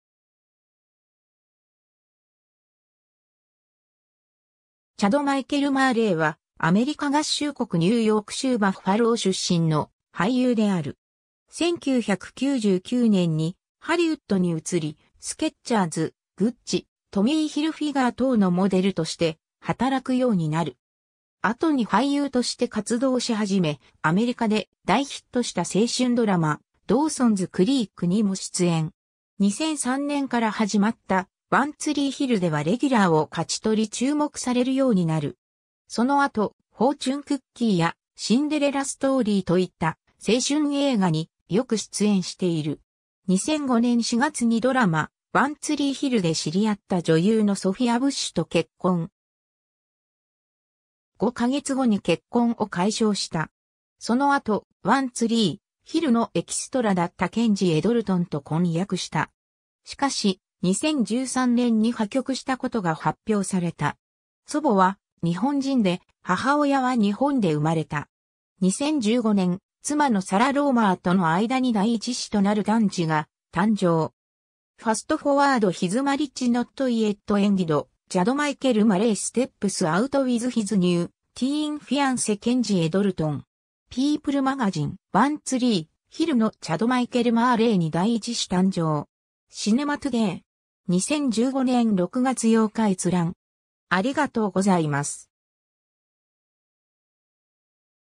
チャド・マイケル・マーレイ
チャド・マイケル・マーレイ（Chad Michael Murray, 1981年8月24日 - ）はアメリカ合衆国ニューヨーク州バッファロー出身の俳優である。
 1999年にハリウッドに移り、スケッチャーズ、グッチ、トミーヒルフィガー等のモデルとして働くようになる。
後に俳優として活動しはじめ、アメリカで大ヒットした青春ドラマ『ドーソンズ・クリーク』にも出演。
2003年から始まった『One Tree Hill』ではレギュラーを勝ち取り注目されるようになる。
その後『フォーチュン・クッキー』や『シンデレラ・ストーリー』といった青春映画によく出演している。
 2005年4月にドラマ『One Tree Hill』で知り合った女優のソフィア・ブッシュと結婚。
5ヵ月後に結婚を解消した。
その後『One Tree Hill』のエキストラだったKenzie Daltonと婚約した。
しかし、2013年に破局したことが発表された。
 祖母は日本人で、母親は日本で生まれた。
 2015年、妻のサラ・ローマーとの間に第1子となる男児が誕生。
 ^ “Fast Forward His marriage not yet ended, Chad Michael Murray steps out with his new, teen fiancee Kenzie Dalton”. People Magazine. (2006年4月24日) ^ “「One Tree Hill」のチャド・マイケル・マーレイに第1子誕生”. シネマトゥデイ (2015年6月7日). 2015年6月8日閲覧。